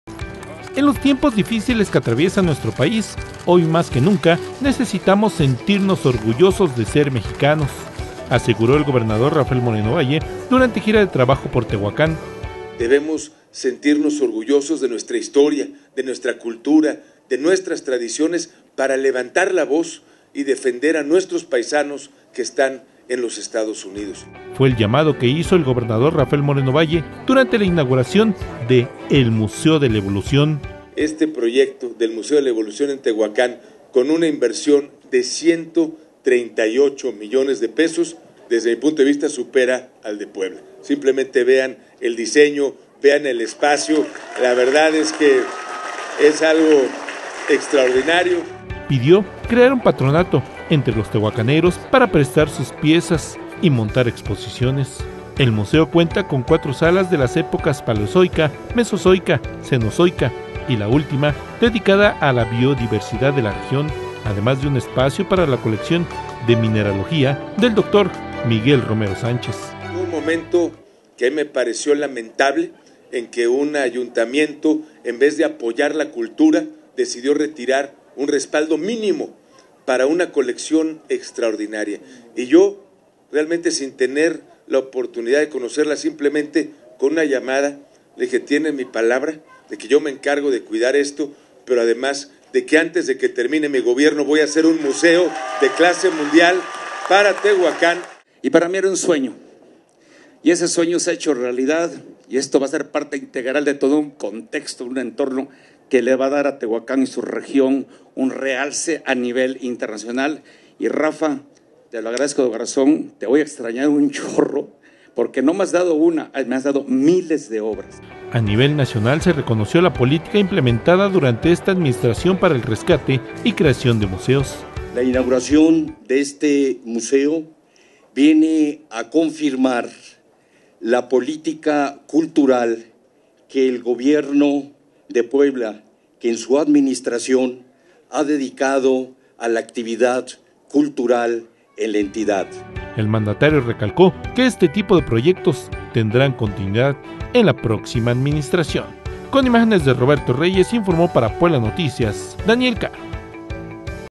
Inaugura Moreno Valle el Museo de la Evolución de Tehuacán
El impulso a la cultura ha sido prioridad de esta administración estatal